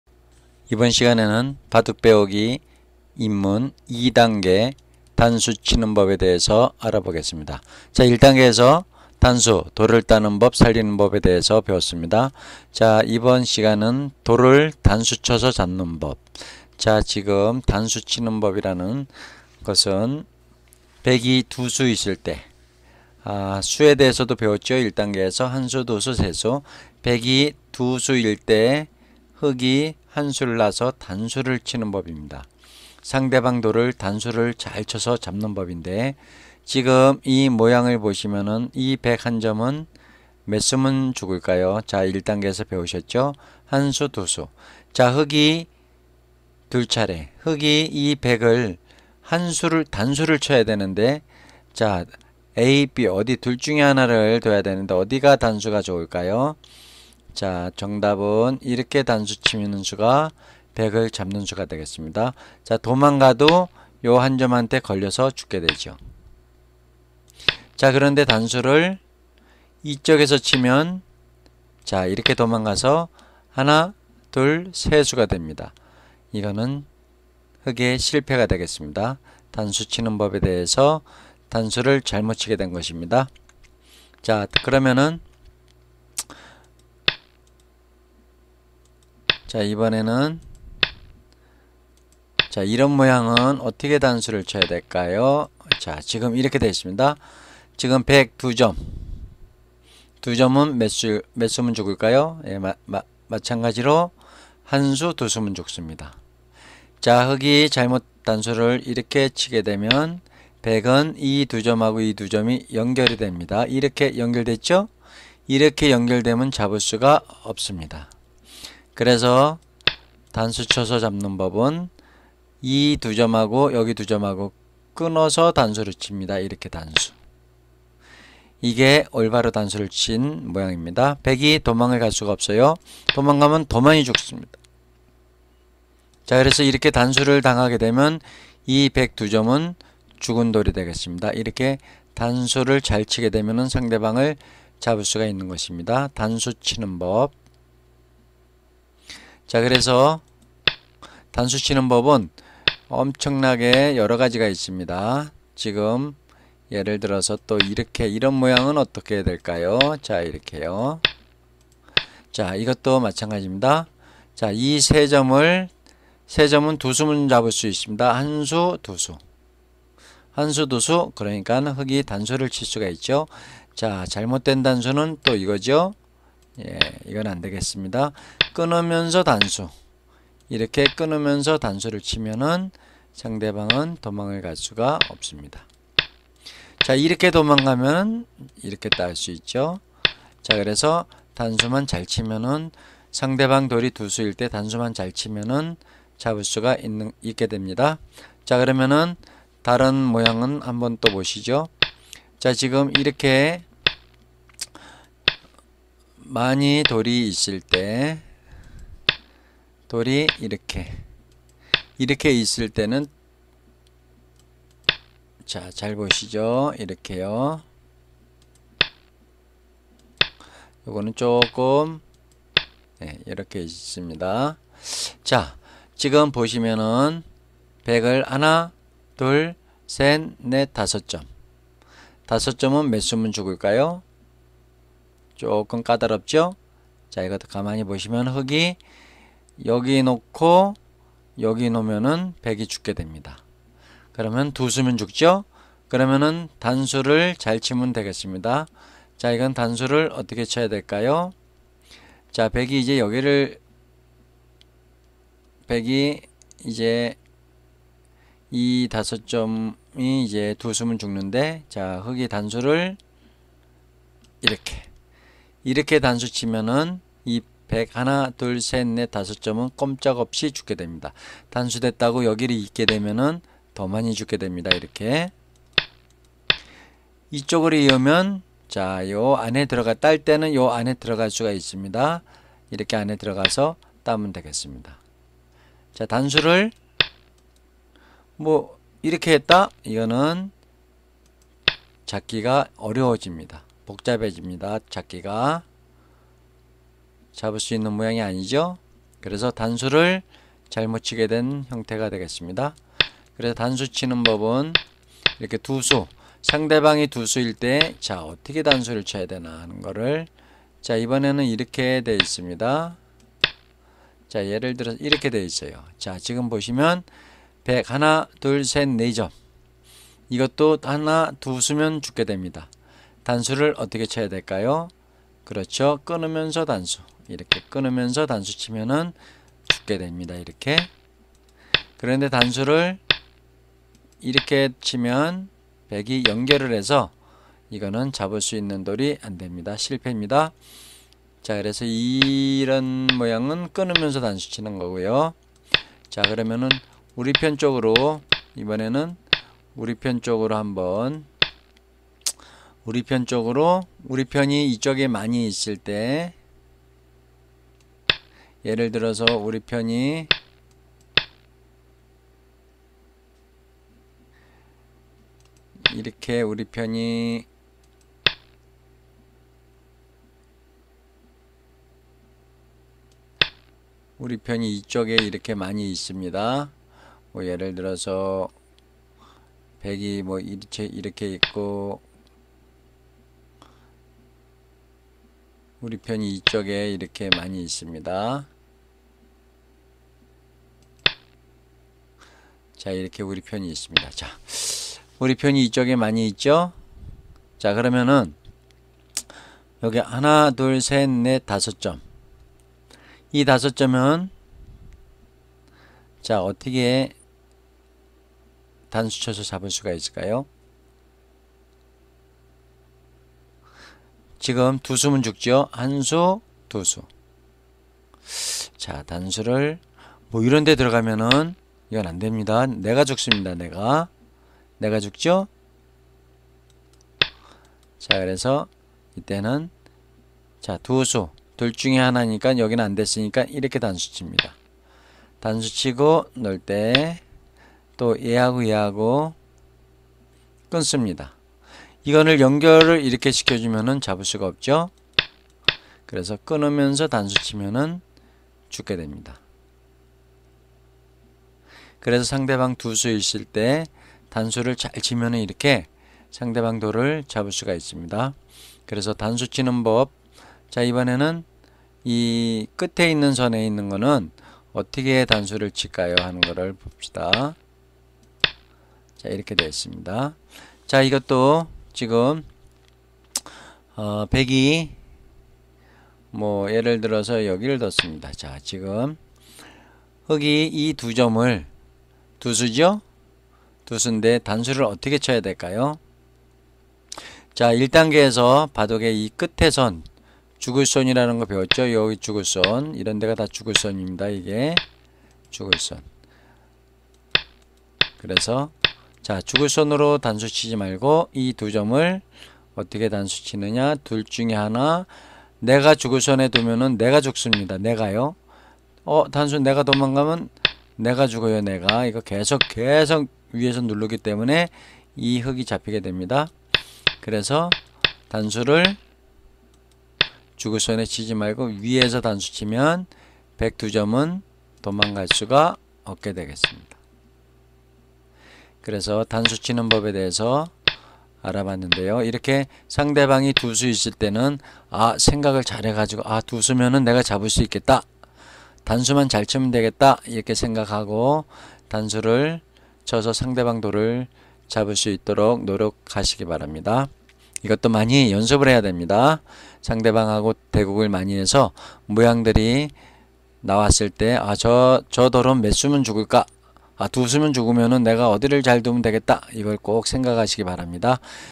단수치는법 ✠ 강욱남 (루치오) 영상편집
13줄 바둑은 배우기도 쉽고 한 판을 20분 안에 끝낼 수 있으니
여러 판을 둬도 크게 시간을 소비하지 않습니다.
치매 예방을 위해 어르신들에게 바둑과 친구가 되게 해 준다고 합니다.